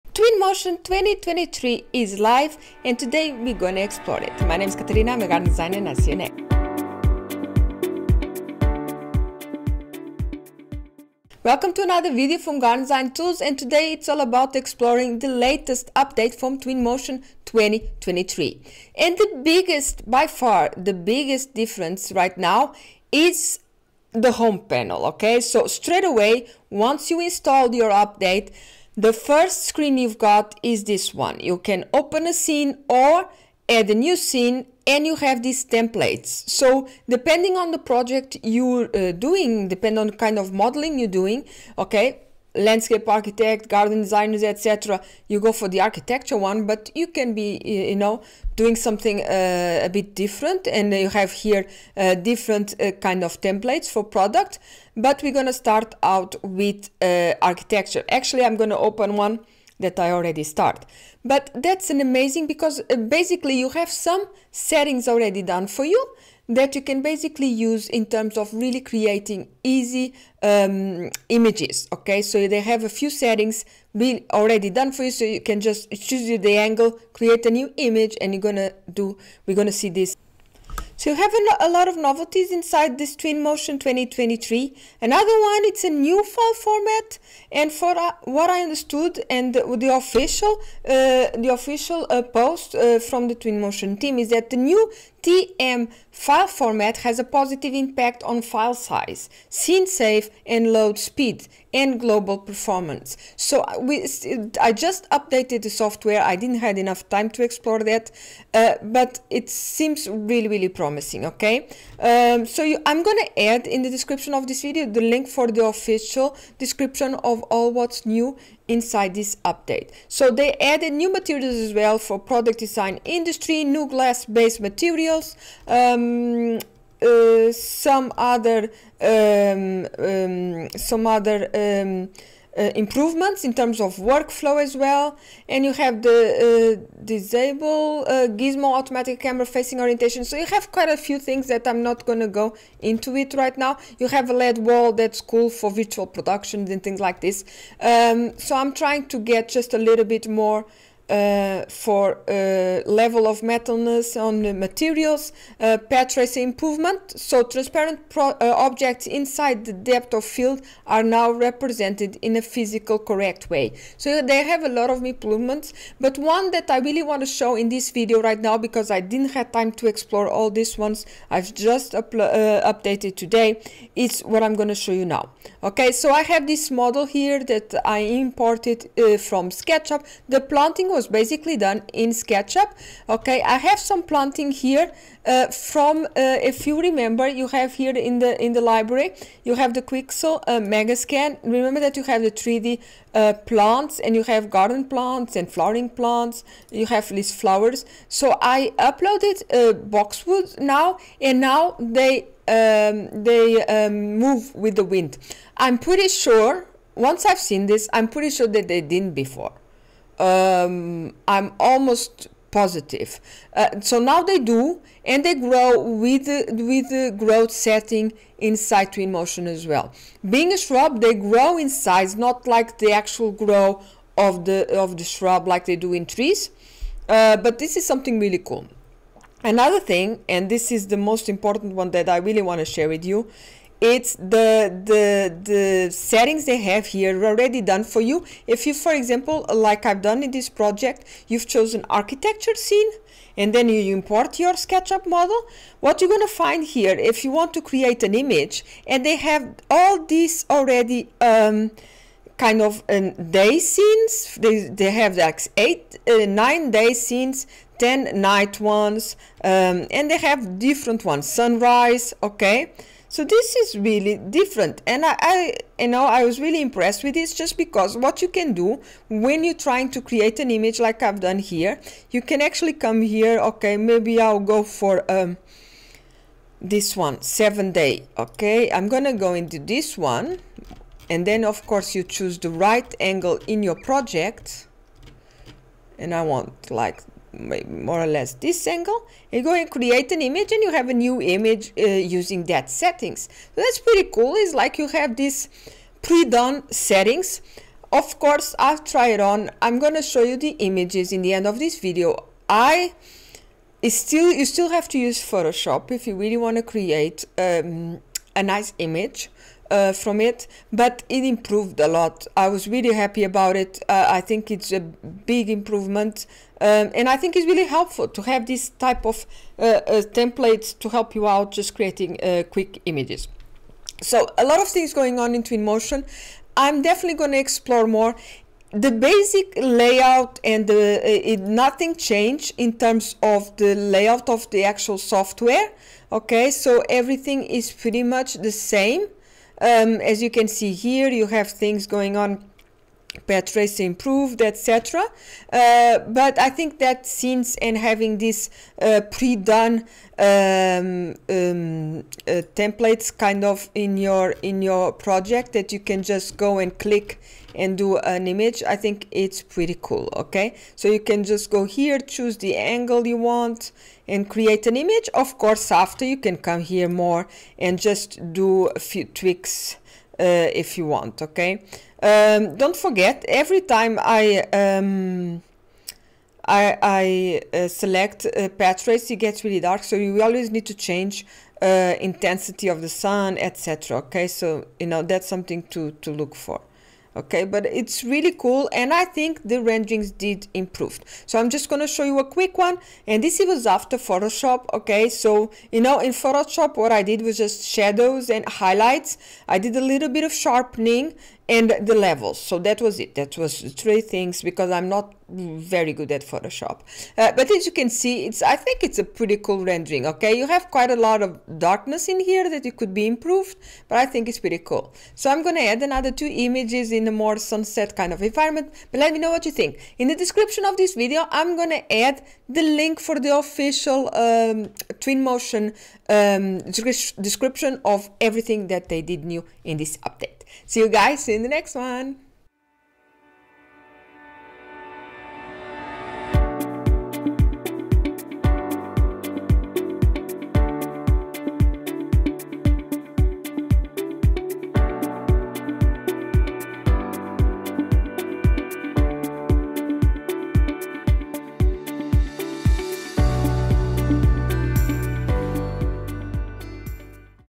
Twinmotion 2023.1 preview now with Templates!!
Amazing new tool inside Twinmotion 2023.1 preview 1 now has Templates, once you download the update you come upon a dashboard where you can choose the template for your project.
Check it out with me in this video. More videos coming soon focusing on the newest updates.